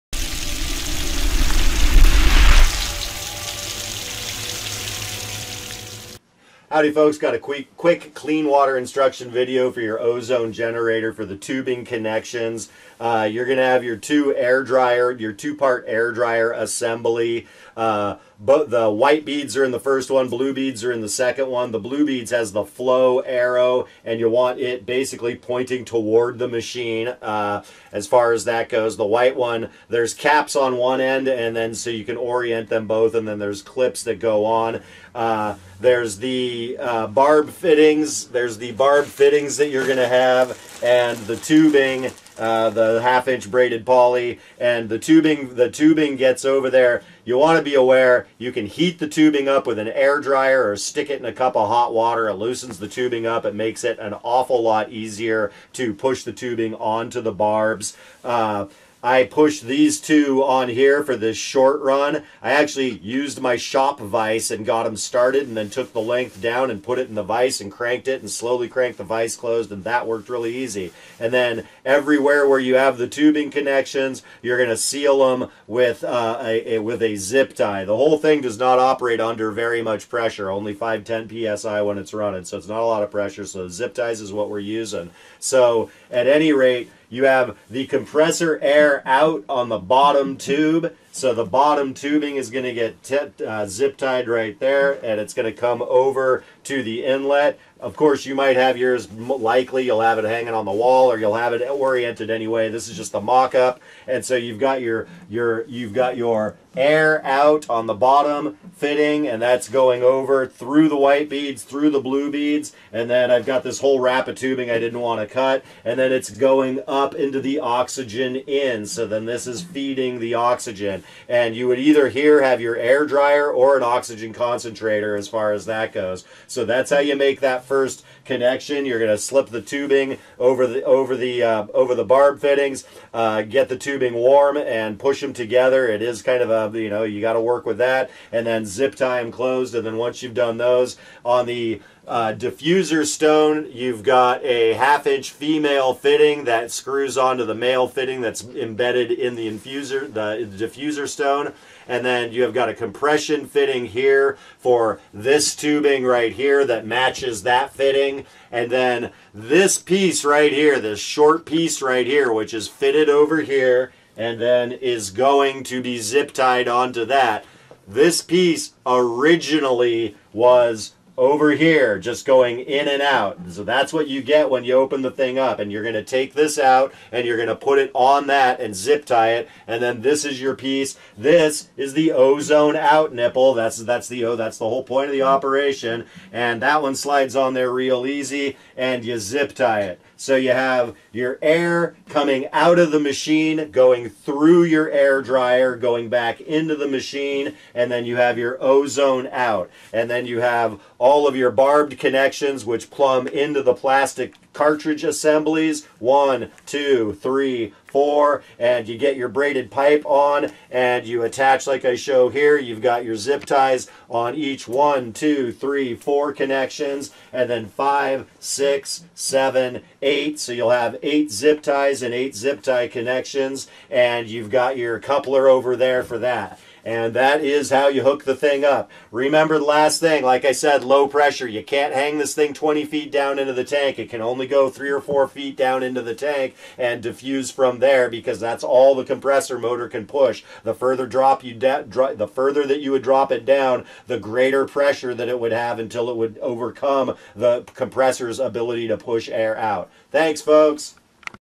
Ozone Dryer Connections
Describes how to connect CWS Ozone 5 Gram/Hour Ozone Generator to Cartridge Type Dryers